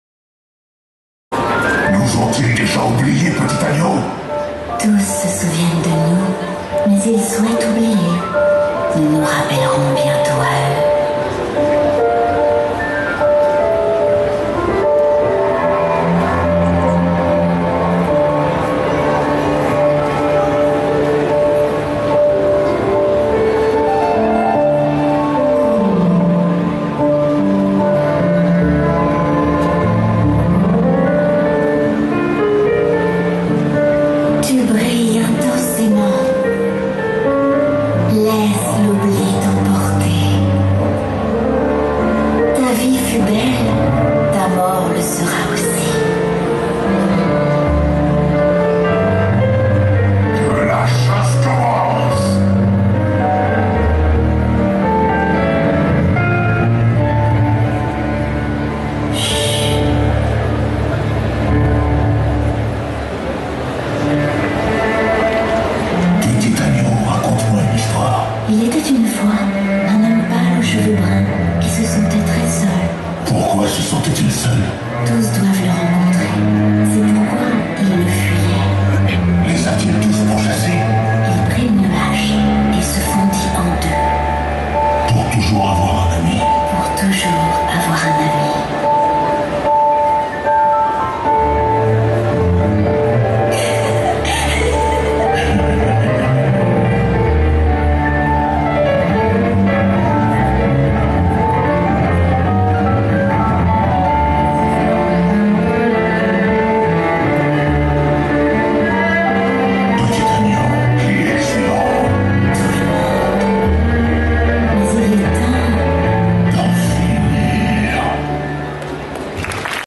08 - ART TO PLAY 2015 - Concours Cosplay
Prestations filmées en toute légalité et avec un accord verbal de l'organisateur.
Ces vidéos ont été capturées gratuitement par notre photographe/vidéaste Pierre Champion.

Concours Cosplay Art to play, organisé par Bullejapon.